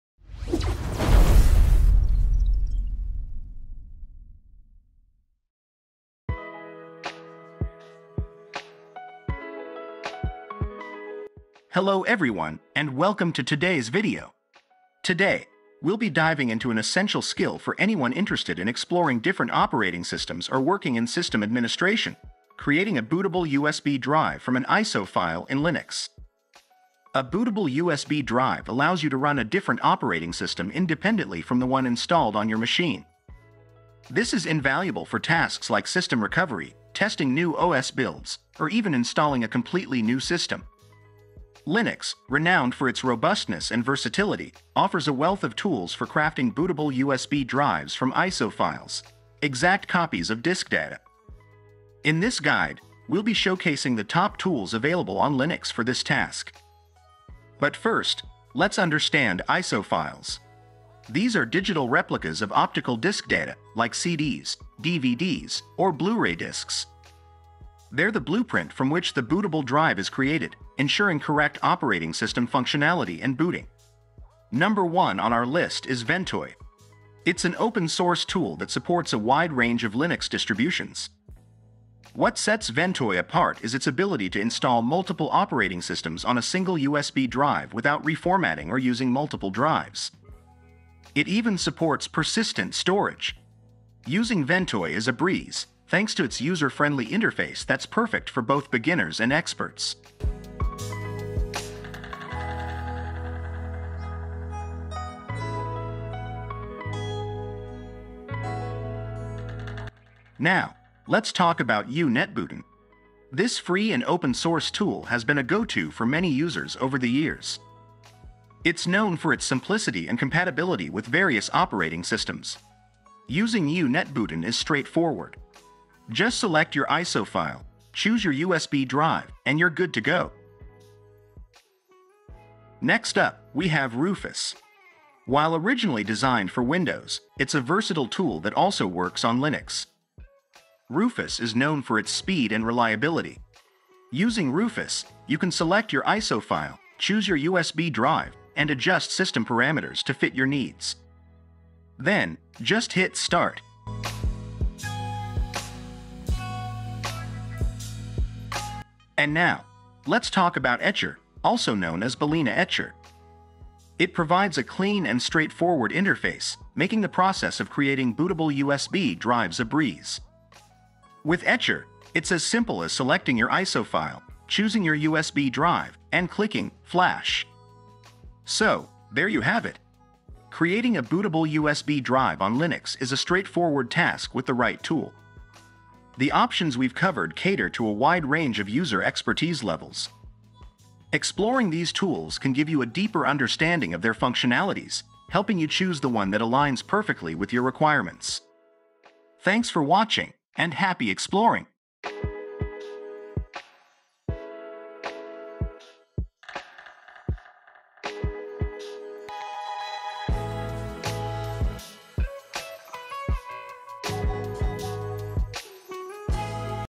Best Tools to Create a Bootable USB in Linux!
Hello everyone, and welcome to today's video! Today, we'll be diving into an essential skill for anyone interested in exploring different operating systems or working in system administration - creating a bootable USB drive from an ISO file in Linux.
A bootable USB drive allows you to run a different operating system independently from the one installed on your machine. This is invaluable for tasks like system recovery, testing new OS builds, or even installing a completely new system.

1. Ventoy
2. UNetbootin
3. Rufus
4. Etcher (BalenaEtcher)